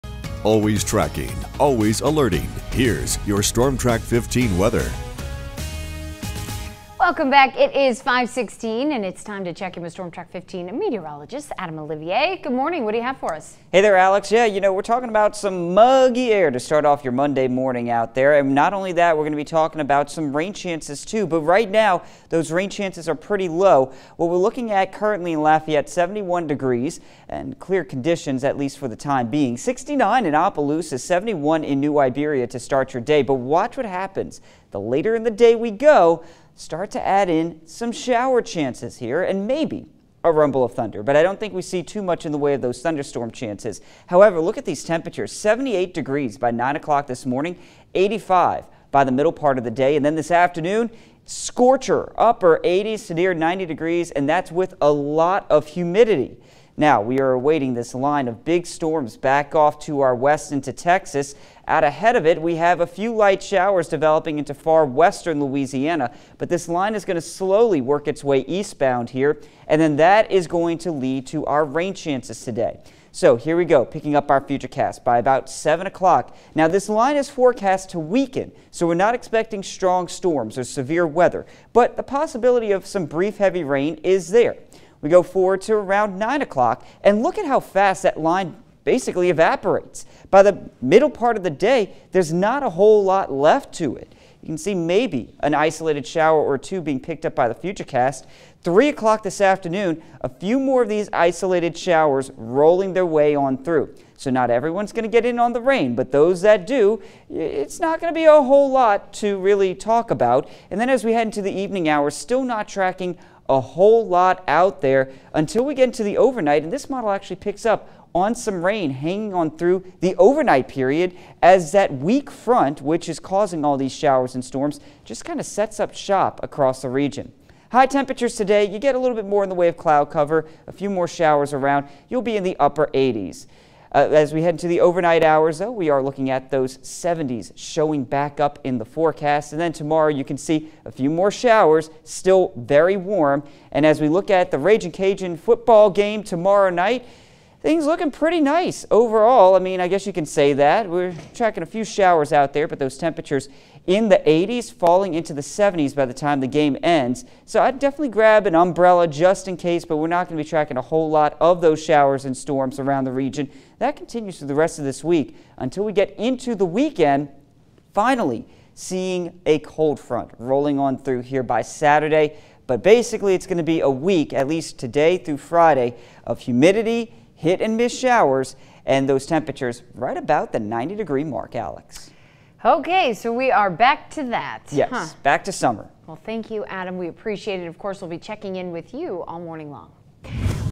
Passing Showers Today
A few showers will roll through the area today.